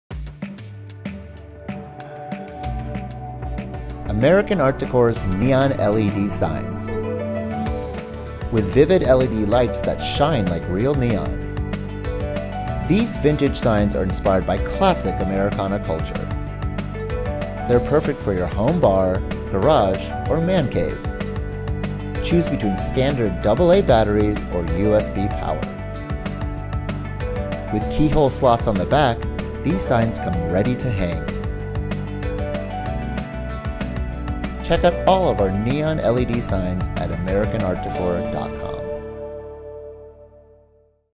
Vintage Shelby Cobra Marquee LED Light Up Sign | American Art Decor
Pay homage to classic car culture with this LED neon light sign that features the Shelby Cobra logo.

Guaranteed to help complete the look in your man cave, garage or auto shop, this vintage-looking sign will surely be a conversation starter and perfect barometer to test the real car enthusiasts in your life.

The vibrant red LED light around the border of the sign really makes it pop, while the black and white text and logo against the red background adds great contrast.

Using LED technology, this sign is made to look like real neon but without the hassle, cost, or loud hum.

A perfect gift idea for Father’s day, dad’s birthday, or for anyone that loves classic cars, this LED neon light sign will surely captivate attention and shine bright!

Whether you use it to decorate your home bar, man cave, garage, or bedroom, this sign looks good anywhere. Comes with 6’ USB cord and plug for any standard electrical outlet. Also operates on 4AA batteries (not included) but we recommend using the USB cord due to limited battery life.

Dimensions: 9.5" H x 17.25" L x 1.5" D
Contains 1 LED sign with USB charger
2 keyhole slots for hanging

VINTAGE SHELBY COBRA WALL DECOR – Embodying the classic Shelby Cobra car logo from days of yesteryear, this light-up sign really has vintage charm and appeal in a fun, neon-like LED sign.

LED NEON SIGN – This fun, retro-inspired sign looks like real neon but without the loud buzz and blinding light! Outlined with a bold red LED light that gives the sign punch and pizzazz, this sign will surely shine bright and is a cost-effective and environment-friendly use of power.

MAN CAVE WALL DECOR – Whether for your home bar, man cave, game room, garage, or bedroom, this neon-like LED sign looks great in a variety of settings.

BATTERY OR USB-POWERED – Equipped with a 6’ USB cord, you can plug into any outlet to power your sign or choose a cord-free option with 4 AA batteries (not included).

READY TO HANG – Outfitted with large keyhole slots in the back, this sign is easy to hang.